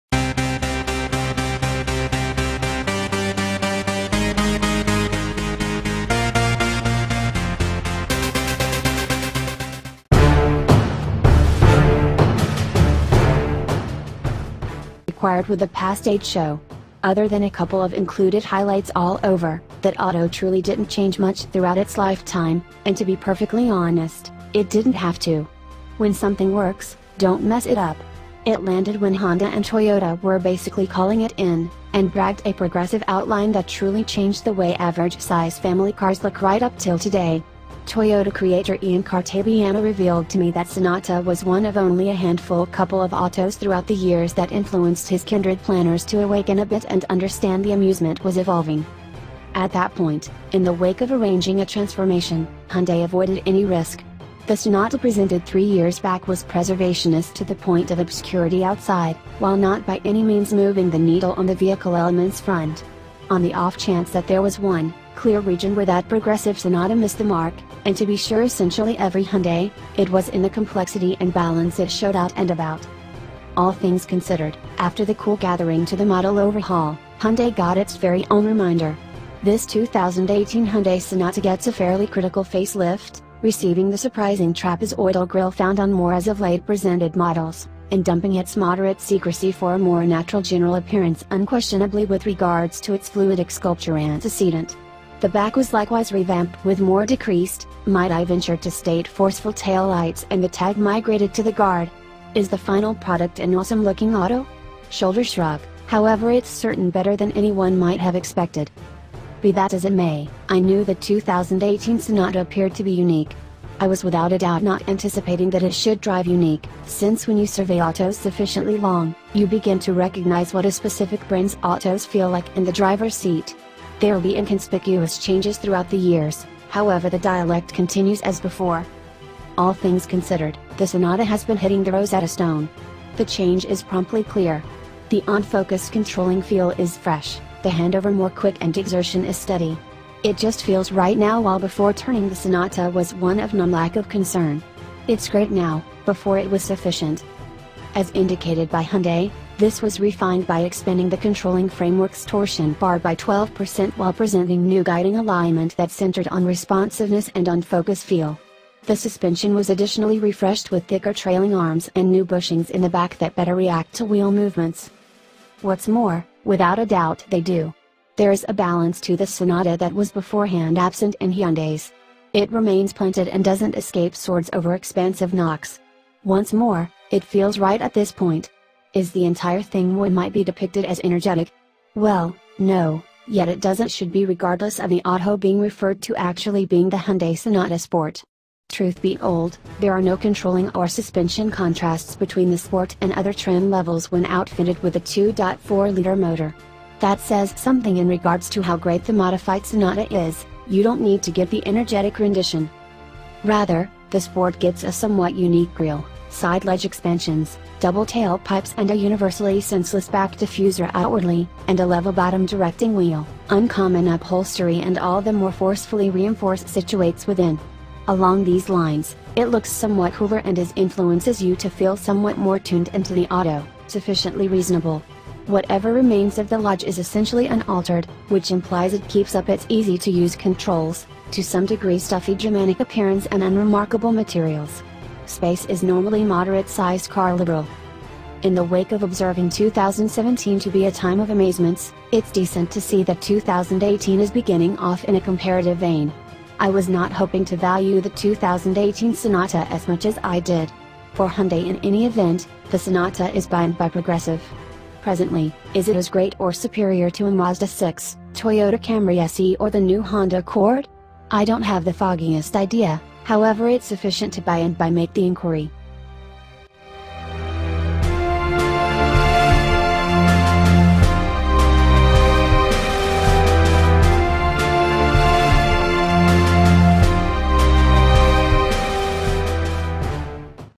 Look This !!! 2018 Hyundai Sonata
Watch Now - 2018 Hyundai Sonata
The 2018 Hyundai Sonata speaks to a course adjustment of the sort that simply wasn't required with the past age show. Other than a couple of included highlights all over, that auto truly didn't change much throughout its lifetime, and to be perfectly honest, it didn't have to. When something works, don't mess it up. It landed when Honda and Toyota were basically calling it in, and bragged a progressive outline that truly changed the way average size family cars look right up 'til today. Toyota creator Ian Cartabiano revealed to me that Sonata was one of only a handful couple of autos throughout the years that influenced his kindred planners to awaken a bit and understand the amusement was evolving. 2018 Hyundai sonata, 2018 hyundai solaris

All things considered, after the cool gathering to the model overhaul, Hyundai got its very own reminder. This 2018 Hyundai Sonata gets a fairly critical facelift, receiving the surprising trapezoidal grille found on more as of late presented models, and dumping its moderate secrecy for a more natural general appearance unquestionably with regards to its "Fluidic Sculpture" antecedent. The back was likewise revamped with more decreased, might I venture to state "forceful" taillights and the tag migrated to the guard. Is the final product an awesome looking auto? Shoulder shrug, however it's certain better than anyone might have expected.
help us to be better.